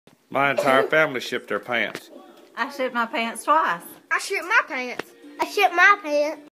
Whole family shipped their pants
It's the holidays and the whole family had their pants shipped in